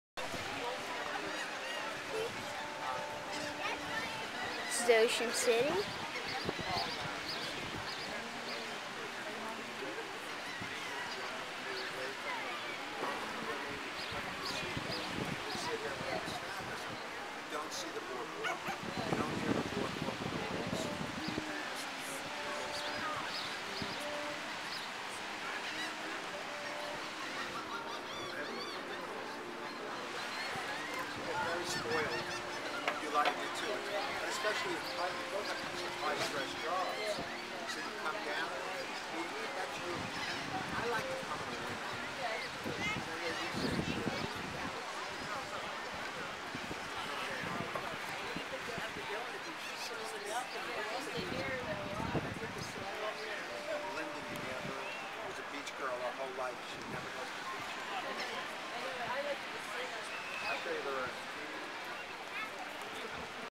Ocean City [Part 2]
this is a nice view of Ocean City taken from a balcony up high.  It's one of my favorite places in the world.